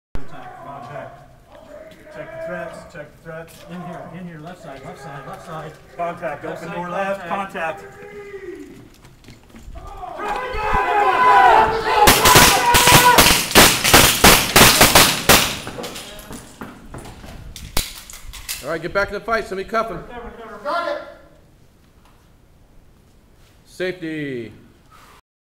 Active Shooter
TAC*ONE Consulting: Active Shooter Training scenario